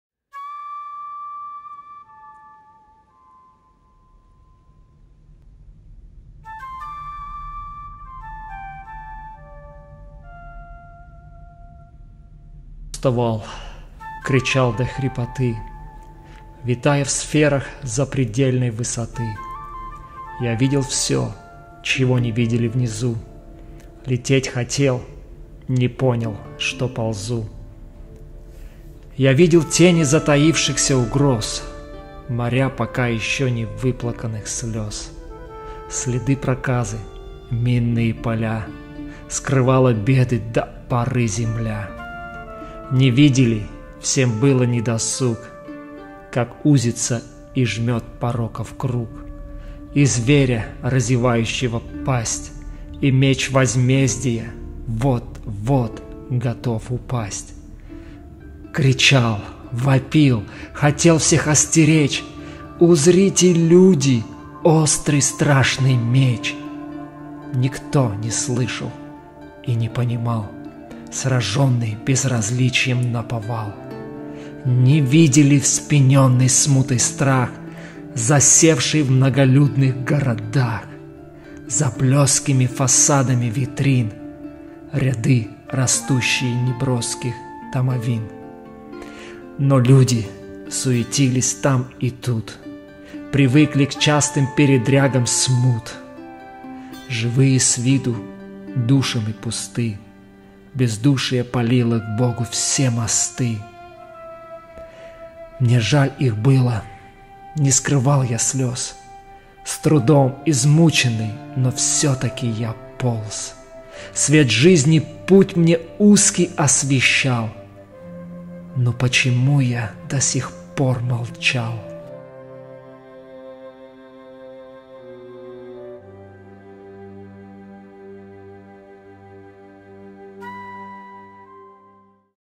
Почему Я Молчал / автор Александр Бутов
Почему Я Молчал / автор Александр Бутов / читает Алексей Бутов